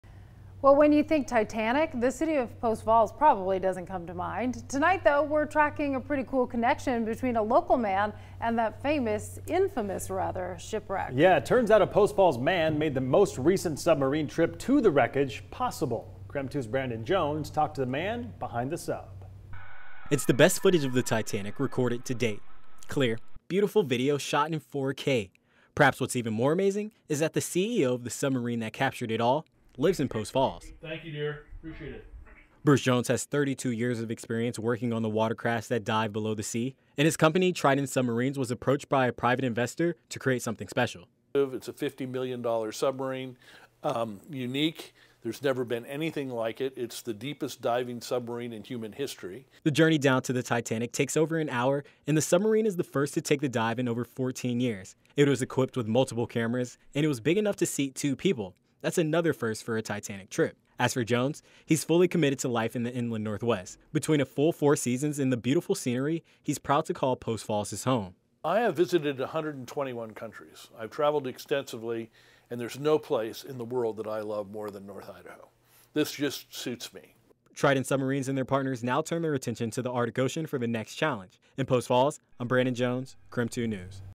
Submarine used in Titanic expeditions designed by Post Falls man
For the last three years, Triton Submarines has traveled to the lowest points of the ocean.